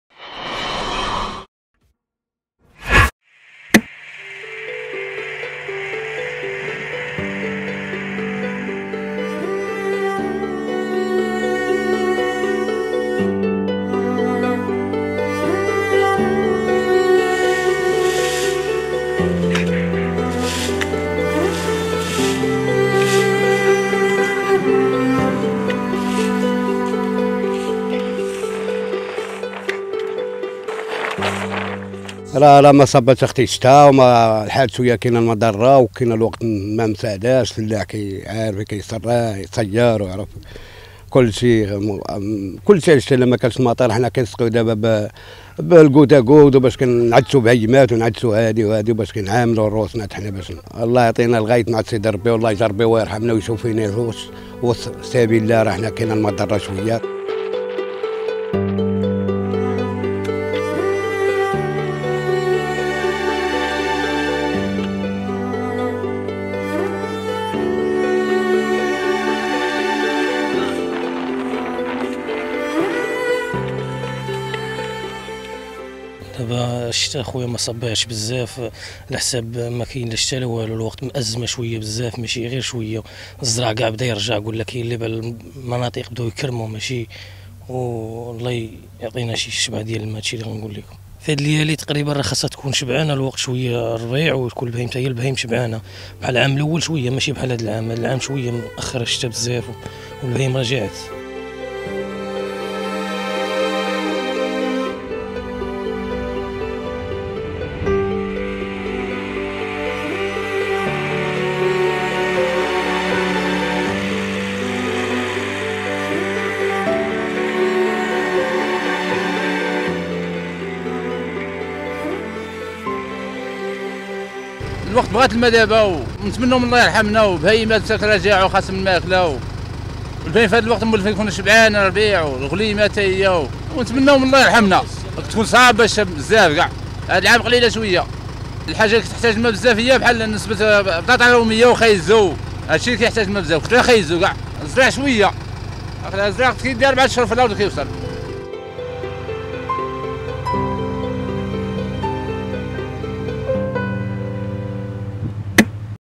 Le360.ma • عيون الفلاحين تتطلع للسماء ومخاوف من قلة التساقطات المطرية
بدأ تأخر التساقطات المطرية يثير مخاوف الفلاحين بجهة الدار البيضاء، حيث تعيش مختلف المناطق الفلاحية حالة من التأهب خوفا من شبح الجفاف. لهذا انتقلت كاميرا le360 إلى منطقة الدروة ضواحي مدينة برشيد، لتنقل لكم أجواء ترقب الفلاحين للأمطار، الذي أضحى تأخرها يشكل كابوسا لهم.